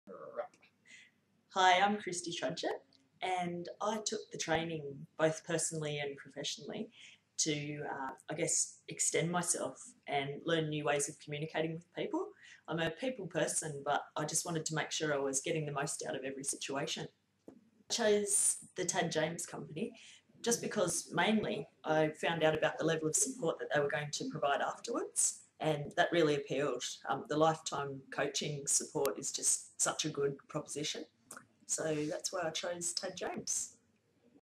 NLP Coaching | The Tad James Co. Testimonials 03
NLP Coaching | The Tad James Co.
Testimonials 03 - Why you should complete your course with the Tad James Co